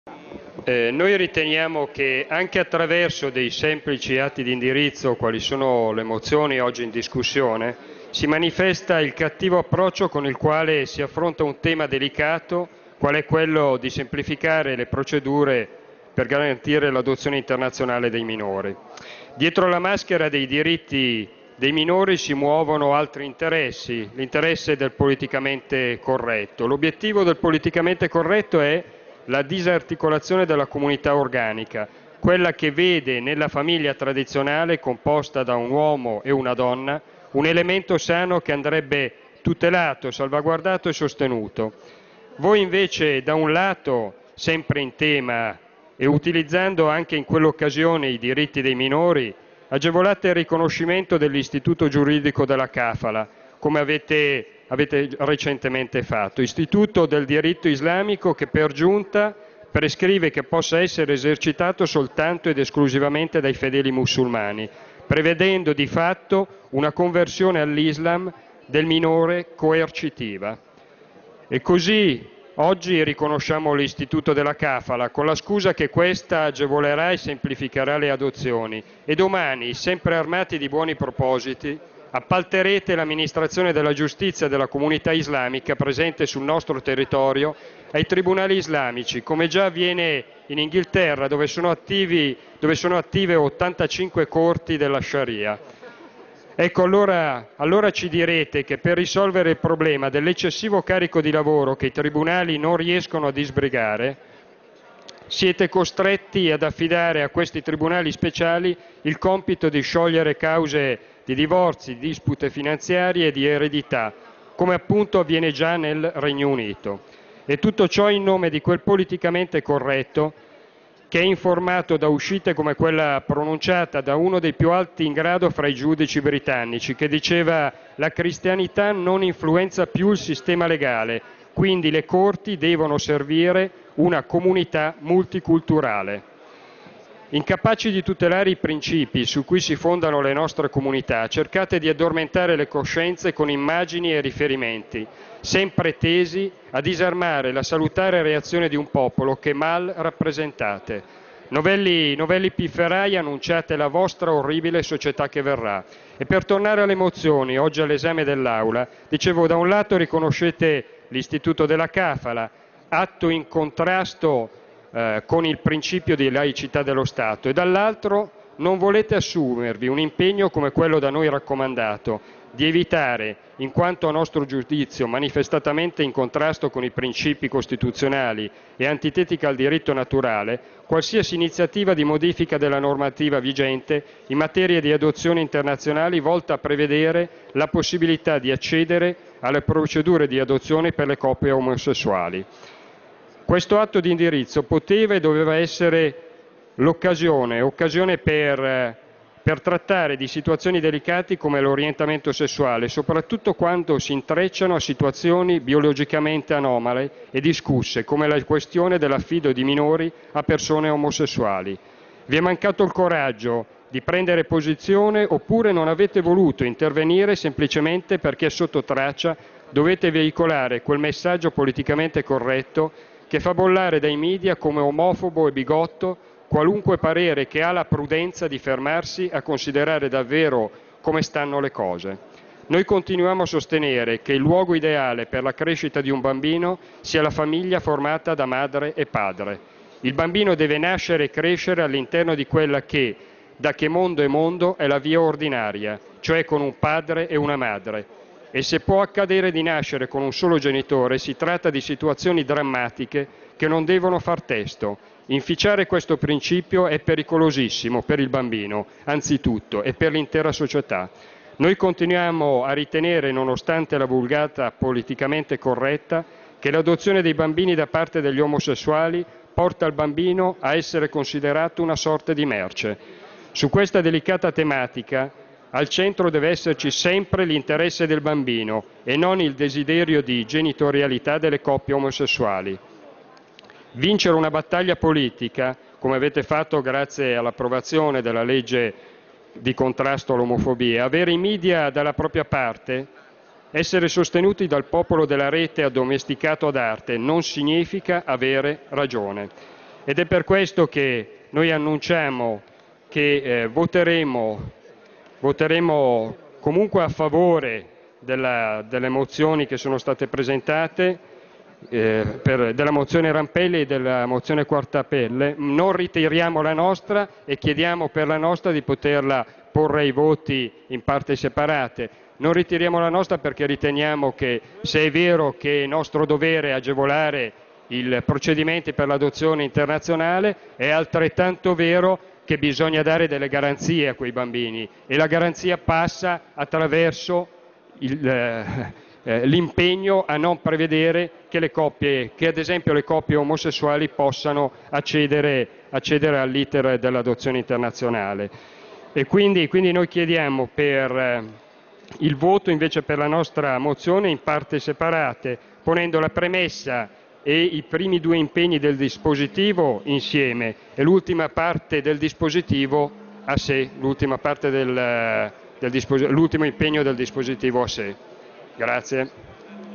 Gay, mozioni lega contro adozioni omosessuali e islam
"No alle adozioni internazionali a coppie omosessuali, né a ingerenze islamiche sulla pelle dei bambini". È in sintesi il contenuto della mozione Lega Nord presentata oggi in aula, ma bocciata da governo e maggioranza. "Non accettiamo che i bambini vengano sacrificati al capriccio di genitori omosessuali o a interessi di parte. Il luogo ideale per la crescita di un figlio è solo e unicamente la famiglia composta da padre e madre", ha detto il deputato Marco Rondini. Nella mozione del Carroccio erano inoltre previsti: la semplificazione delle procedure di adozione, la riduzione dei tempi e la gratuità dell'iter. L'atto prevedeva inoltre un impegno da parte del governo a verificare la compatitibità tra la costituzione italiana e l'istituto islamico della kafala, inserito negli accordi internazionali dell'Aja ratificati dalla Camera. Si tratta di un istituto che nega ai minori il cognome di chi ne assume la custodia e ogni tipo di diritto di successione e legami giuridici con la famiglia di accoglienza. "La sinistra dei benpensanti sta svendendo la nostra società all'Islam, il politicamente corretto minaccia la nostra identità - ha protestato Rondini -. Di questo passo dovremo aspettarci la nascita di tribunali islamici, come già avvenuto in Gran Bretagna. Pd e maggioranza ci stanno consegnando una società orribile".